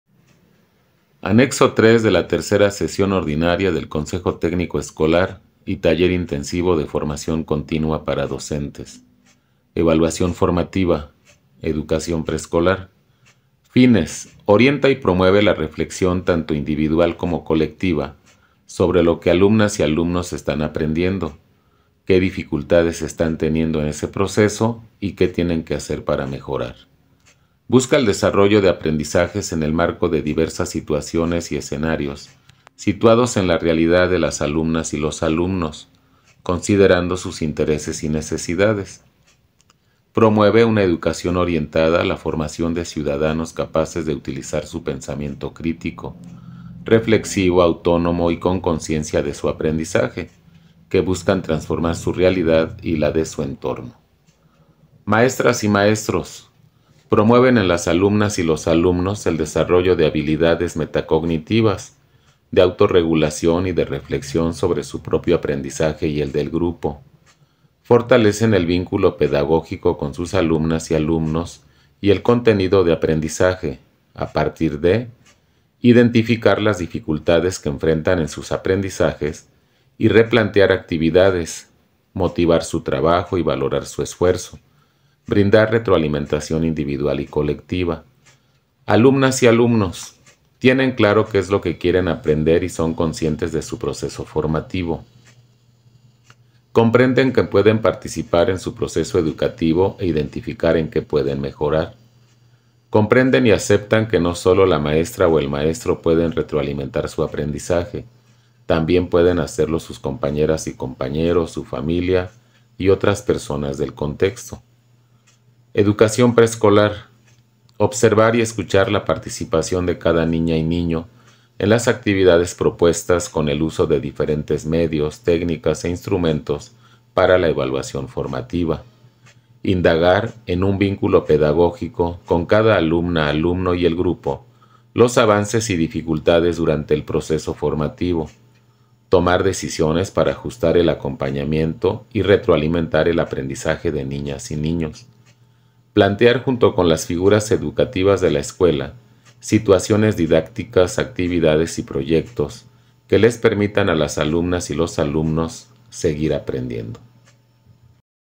Evaluación Formativa | Educación Preescolar | Anexo 3 del CTE
El profesor Alejandro López comparte el contenido del Anexo 3 de la Tercera Sesión Ordinaria del Consejo Técnico Escolar...